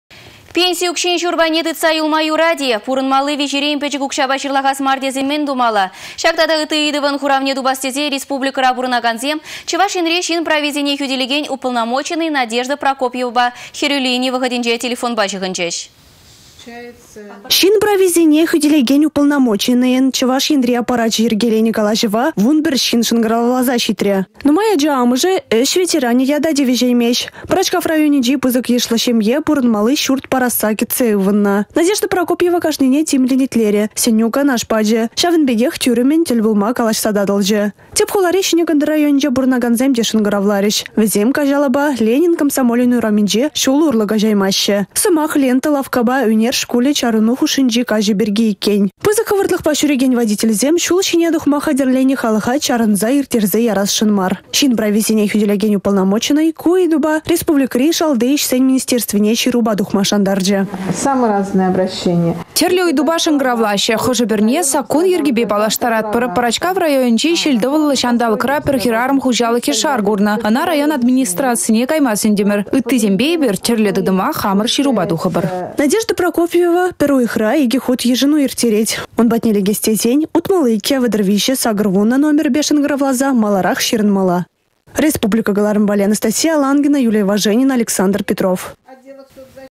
«Хĕрӳ лини»
Пенси укçин çур пайне тытса юлма юрать-и? Вĕсен пурăнмалли виçерен пĕчĕк укçапа çырлахас мар тесен мĕн тумалла? Çак тата ытти ыйтăвăн  хуравне тупас тесе республикăра пурăнакансем Чăваш Енри çын прависене хӳтĕлекен уполномоченнăй Надежда Прокопьевăпа «хĕрӳ лини» вăхăтĕнче телефонпа çыхăнчĕç.